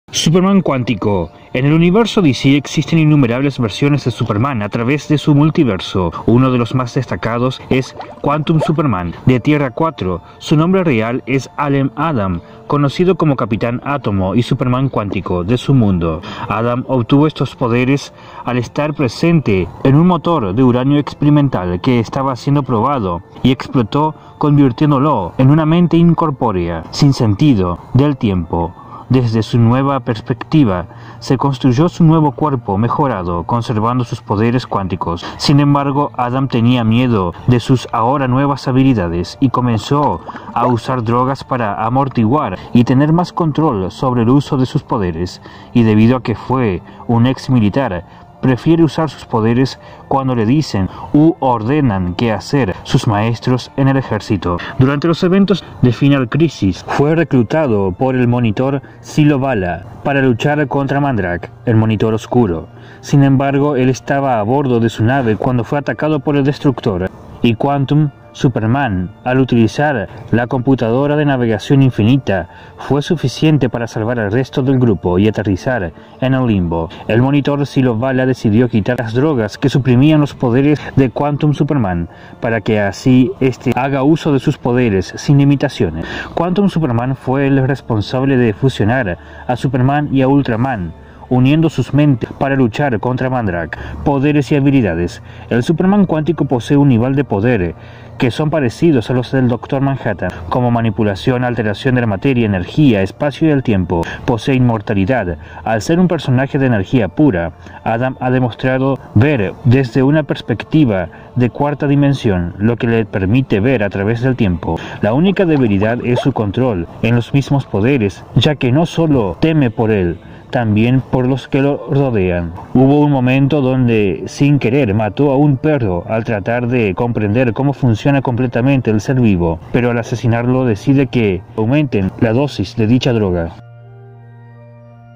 ¿EL DR MANHATTAN TIENE VARIANTES?
Uno de los personajes más desconocidos pero poderosos es el capitán átomo de tierra 4 llamada Superman cuántico